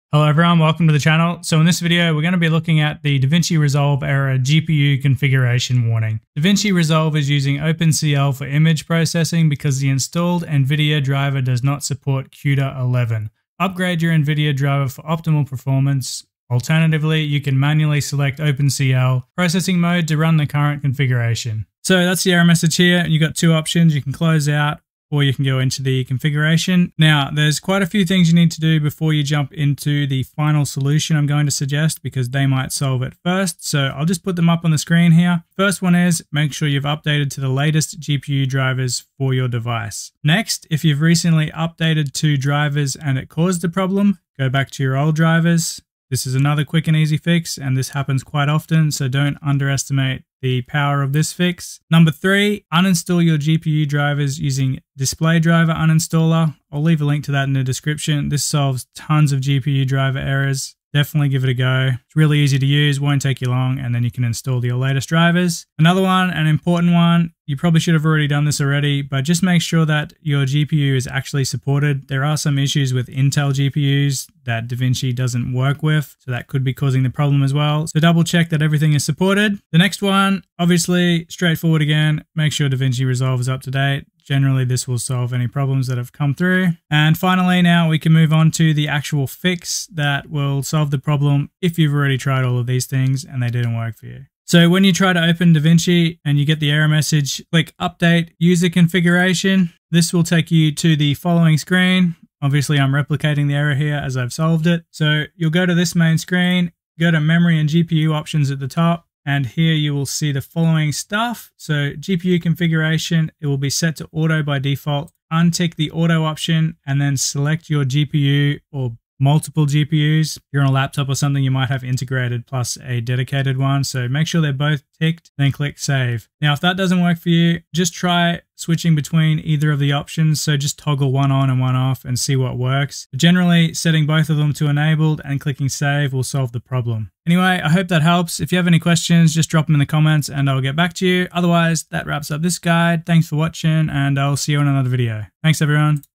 How to Fix the GPU Configuration Warning Error in DaVinci Resolve
If you are getting a GPU Configuration Warning error when opening, rendering, or exporting a project in DaVinci Resolve, this guide will help you resolve the issue. We'll walk you through configuring DaVinci Resolve manually, while also ensuring that your Windows 10 or 11 settings are correctly optimized. Both of these factors can contribute to the error.